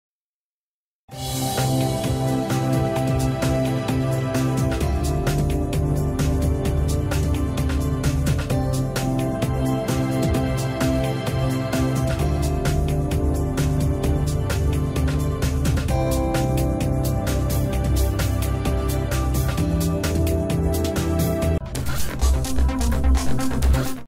J. Balvin abre la última jornada del Festival de Viña y promete encender con su reggaeton
La Quinta Vergara vivirá su última noche de fiesta y contará con el humor de Fabricio Copano y el cierre bailable con Marama y Rombai. Hoy también se conocerá al ganador de la competencia Internacional.